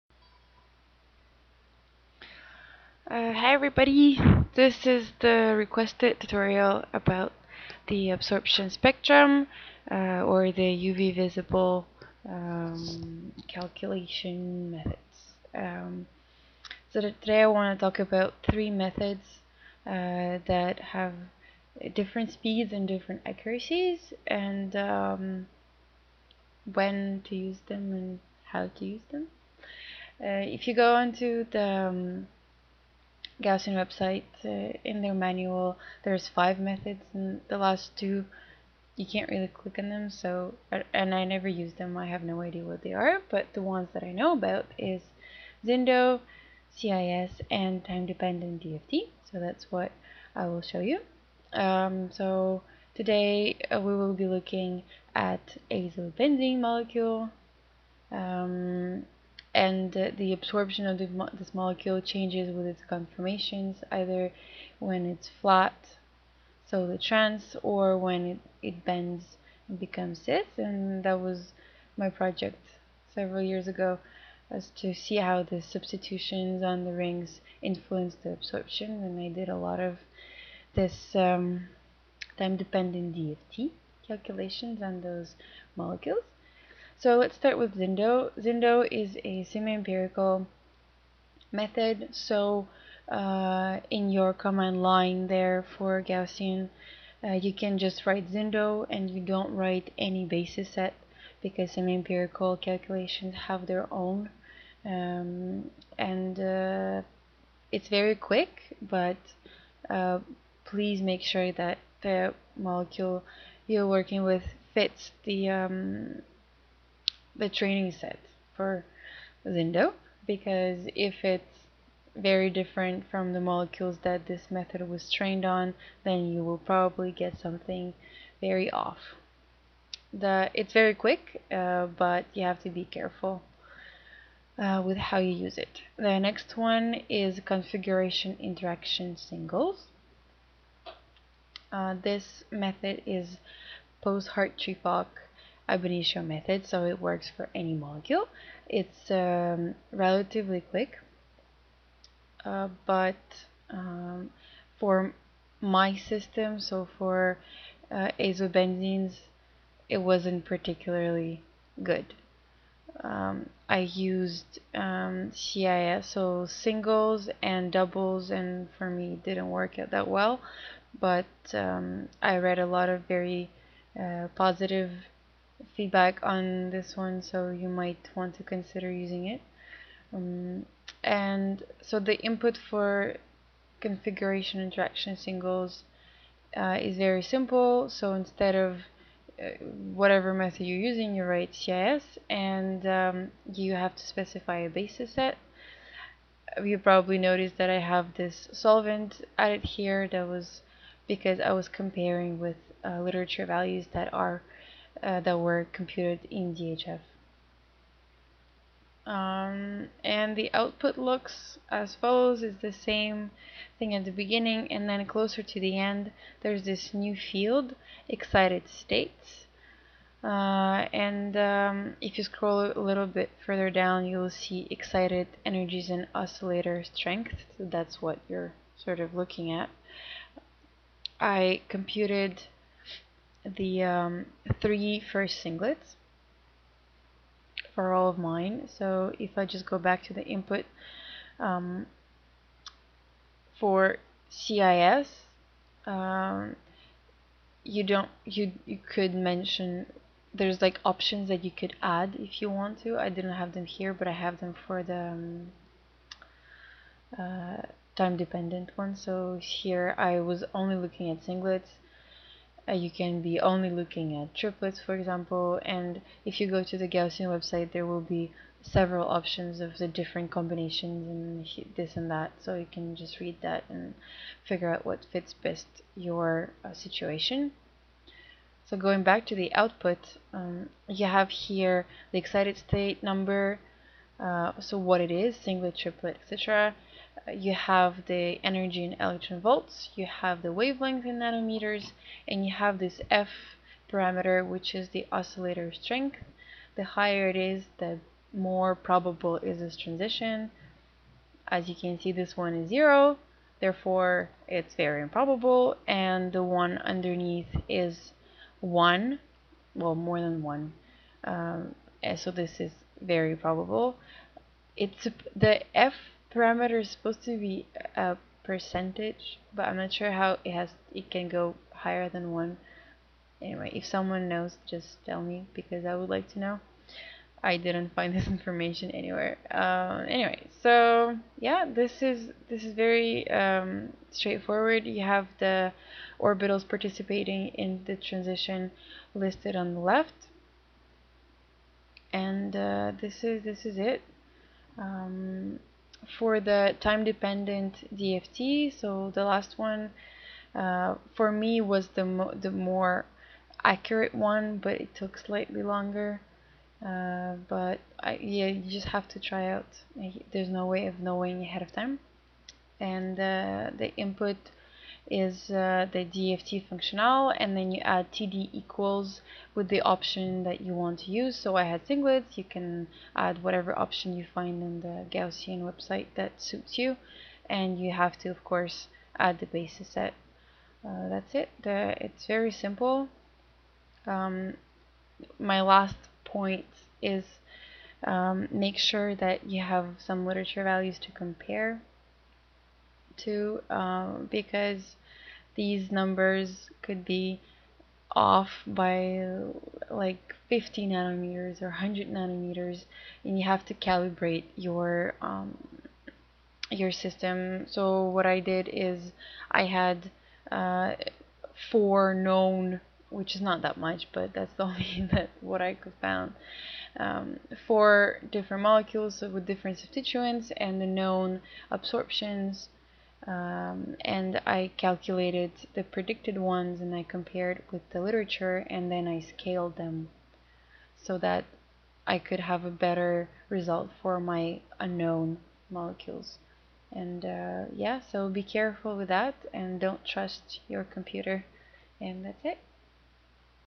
Avogadro with Gaussian Tutorial Absorptions (UV-Vis)
How can you calculate theoretical absorption wavelengths for your molecule? Use ZINDO, CIS(D) or TD DFT! Don't forget to optimize the geometry first!